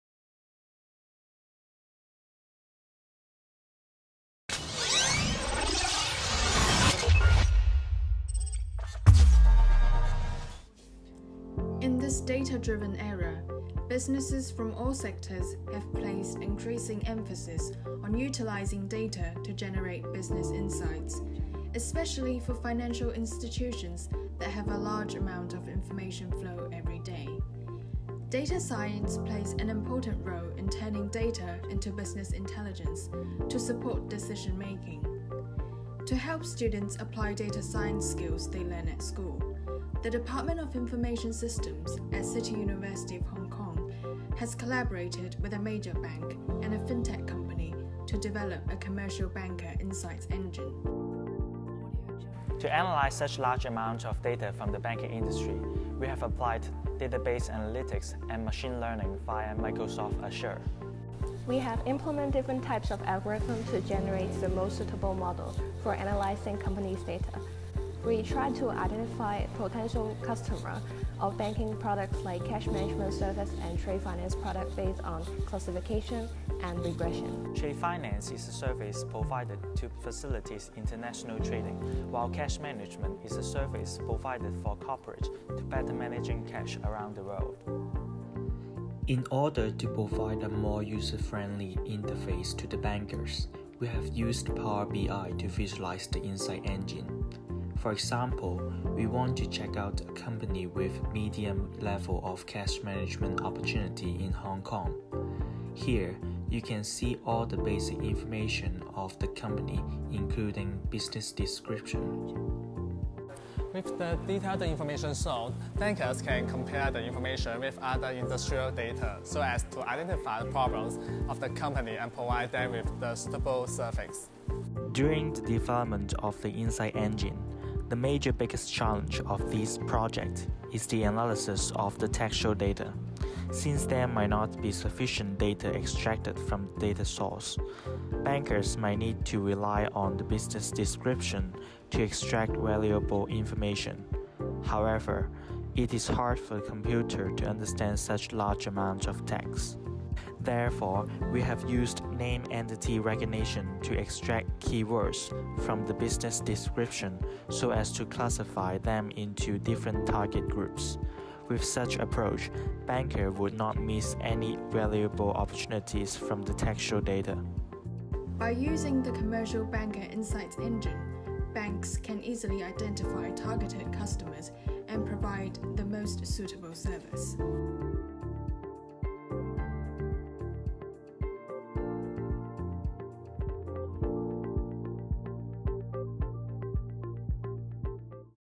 Data Science for Commercial Banking
In this data-driven era, businesses from all sectors have placed increasing emphasis on utilizing data to generate business insights, especially for financial institutions that have a large amount of information flow every day. Data Science plays an important role in turning data into business intelligence to support decision making. To help students apply data science skills they learn at school, the Department of Information Systems at City University of Hong Kong has collaborated with a major bank and a FinTech company to develop a Commercial Banker Insights Engine.